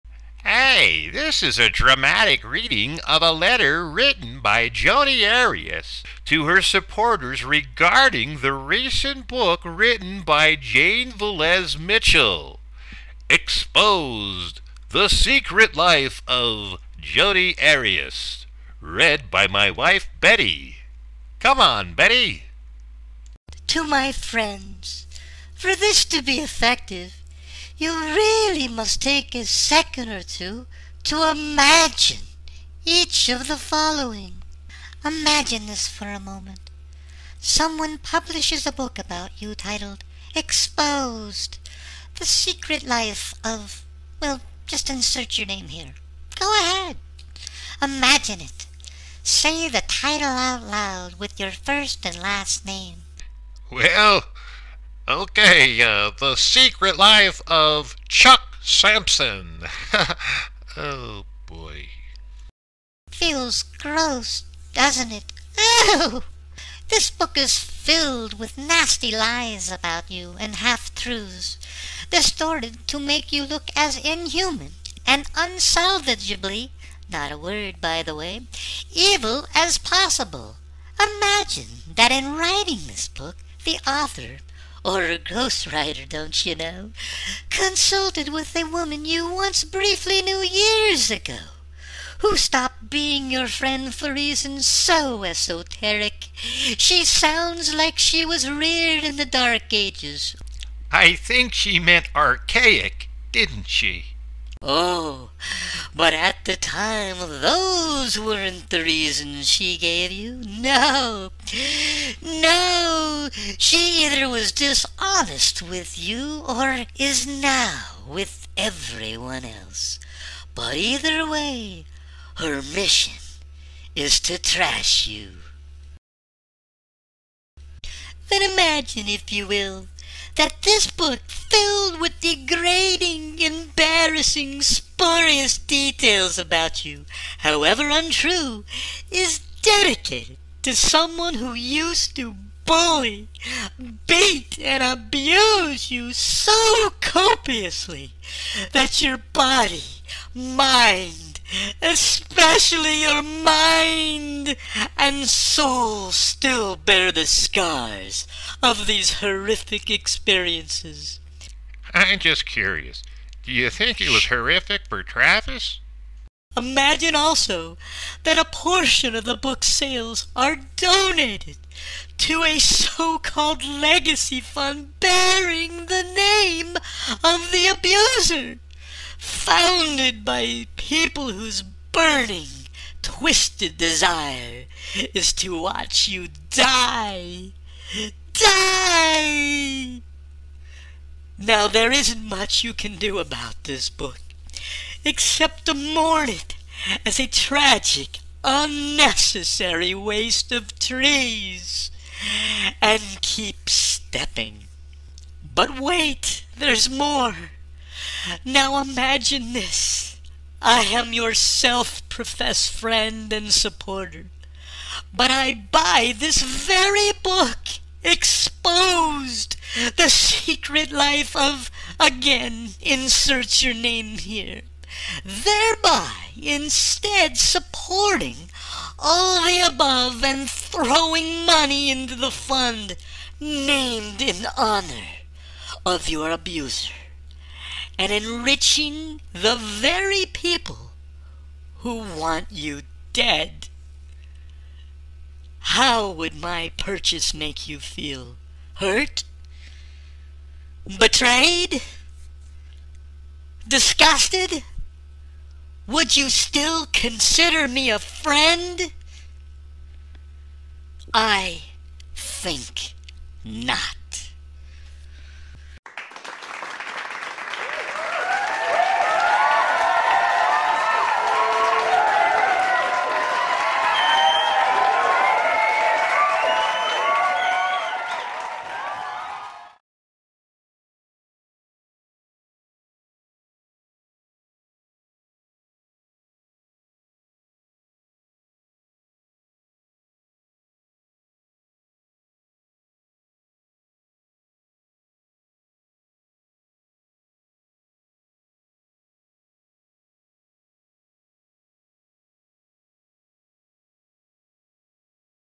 Jodi Arias Letter to supporters
This is a dramatic reading of a letter that Jodi Arias wrote to her supporters about the book that jane velez mitchell  wrote called Exposed, the secret life of Jodi Arias.